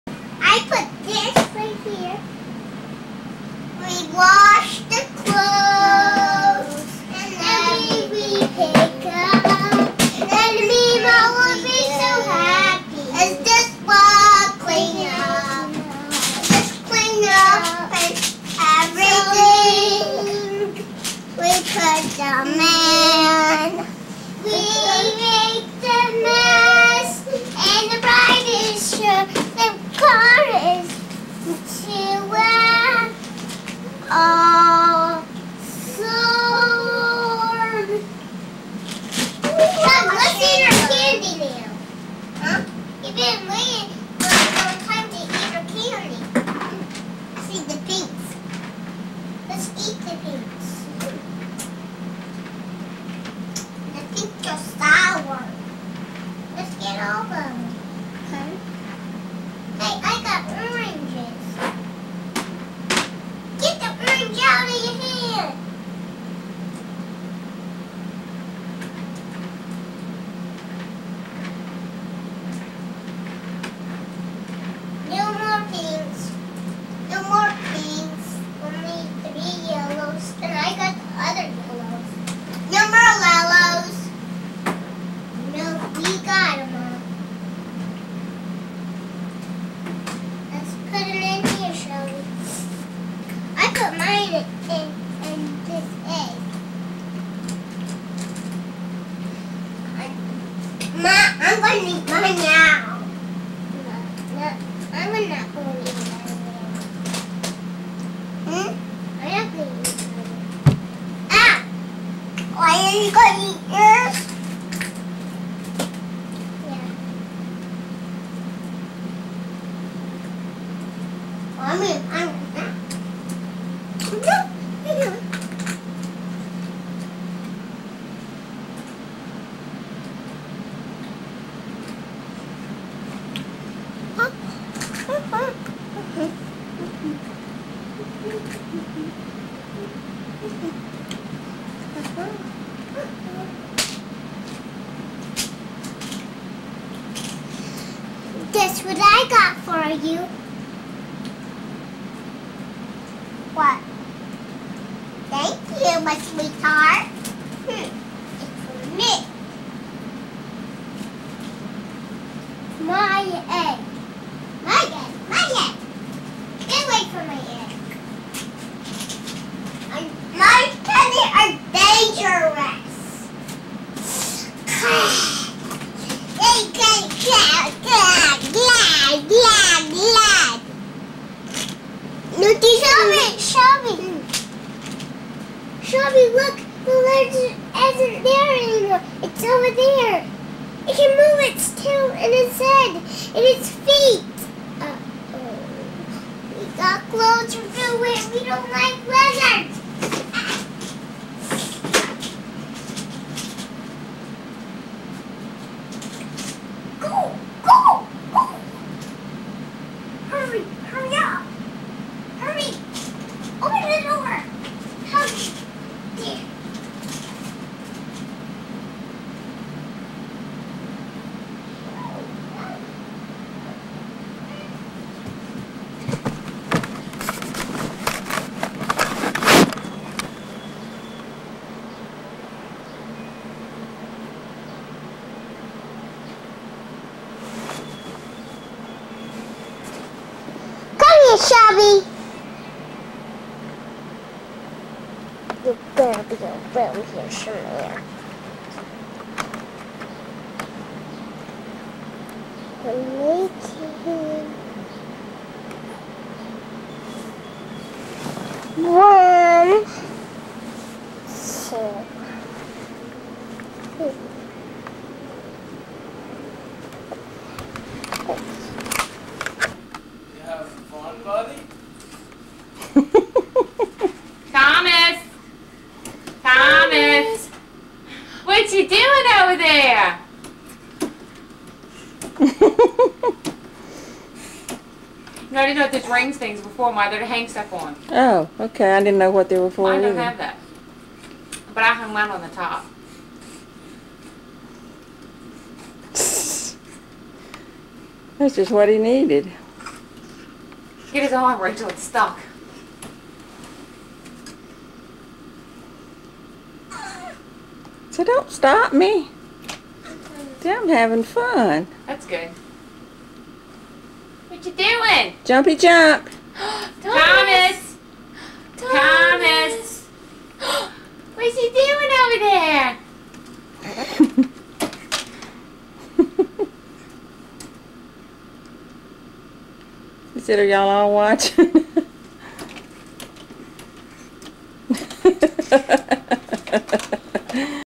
Haylee Mazzella Part 2 Surprise Part to Haylee Films Herself & Shelby
Again, these are tiny little kids trying to film with a video camera, so there's pretty much no picture!  In this part they sing a really cute little song they made up, though.  Haylee & Shelby.